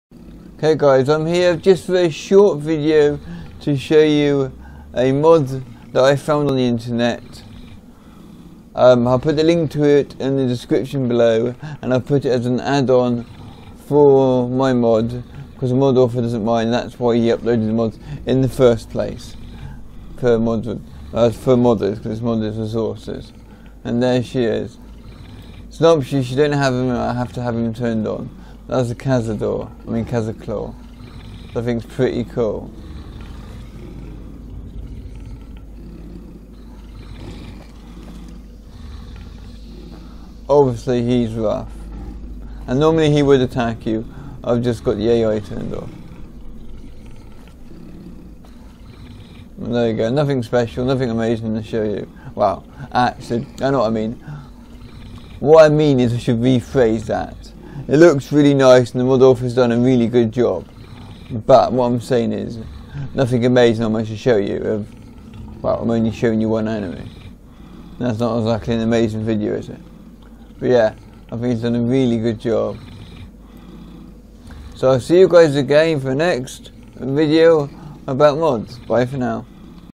Unforgiving Wasteland Optional file Cazaclaw
The optional files adds 4 Cazaclaw all thw works go's to MM

Can anyone please help me with this geck problem thank you